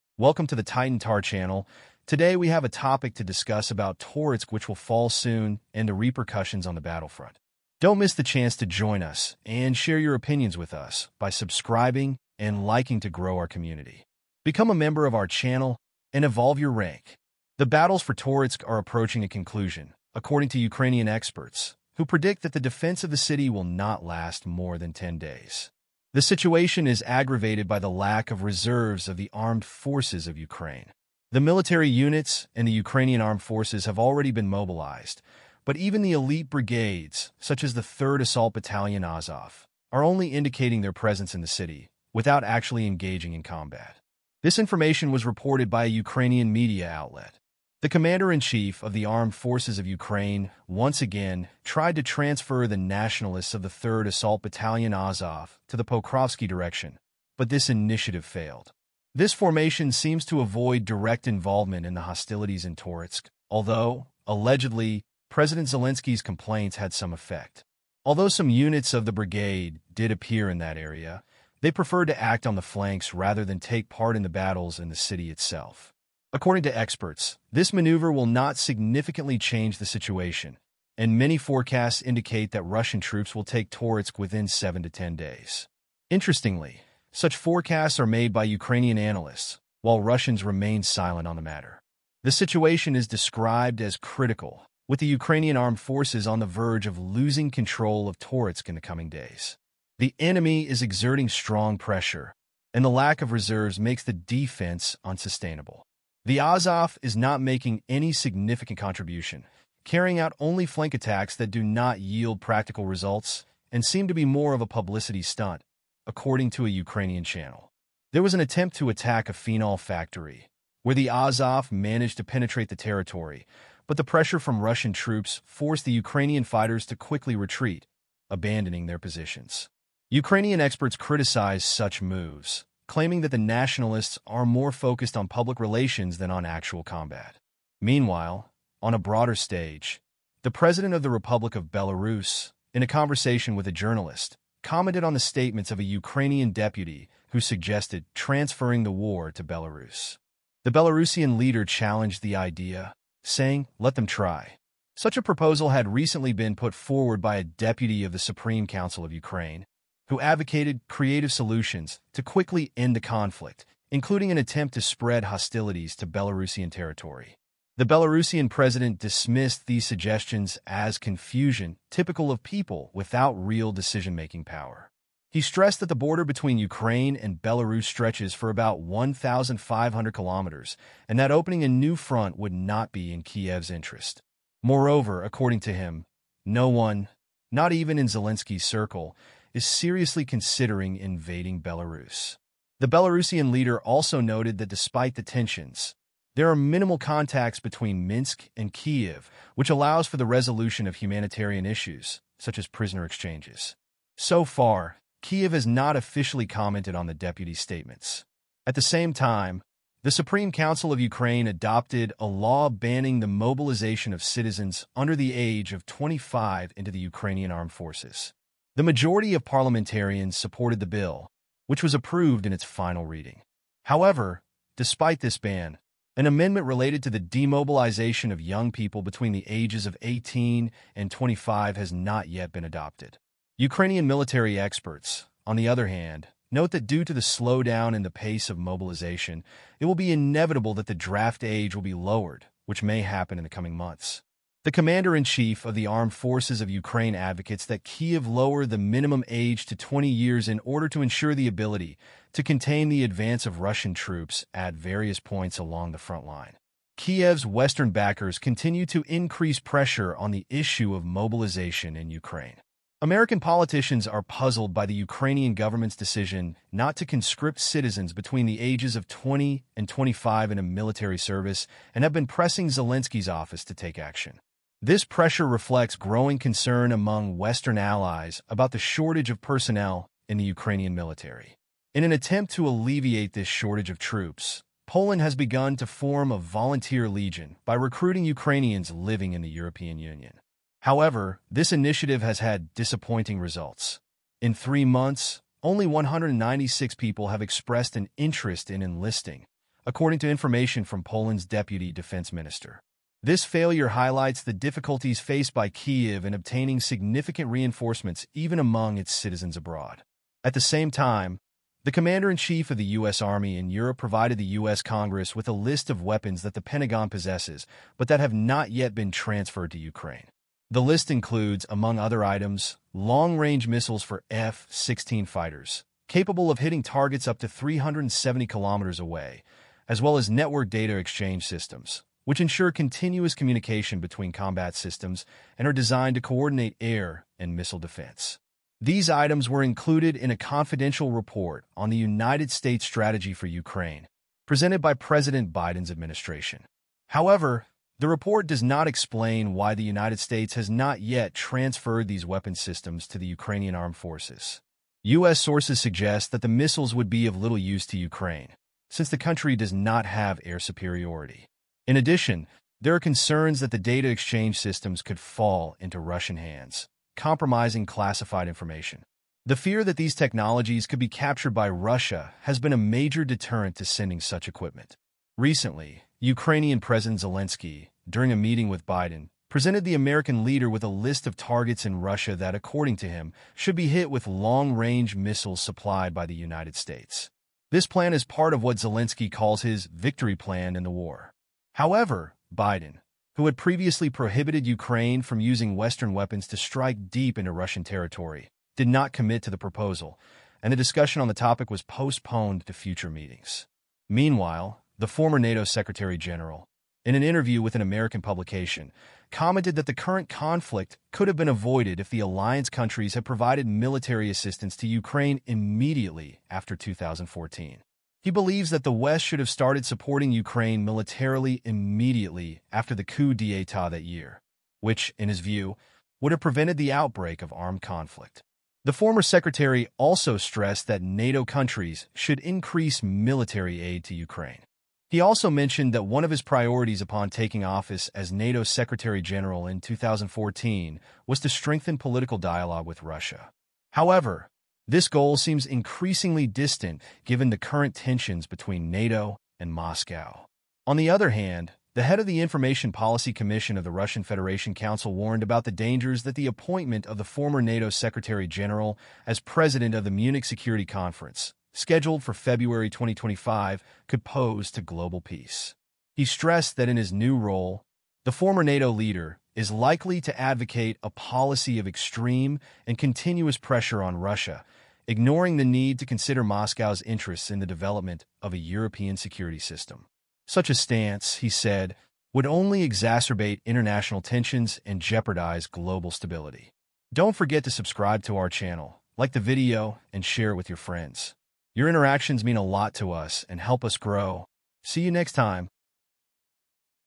Total Control: The Advance of Russian Forces in the Snagost River Bend after Sudzhansky!
In today’s episode of Titan Tar, we dive into an in-depth analysis of the recent advance of the Russian Armed Forces in the bend of the Snagost River, resulting in the recapture of the settlement in the Sudzhansky District. Join us as we unravel the ramifications of this important military development.

Titan Tar, your trusted guide on security and geopolitics!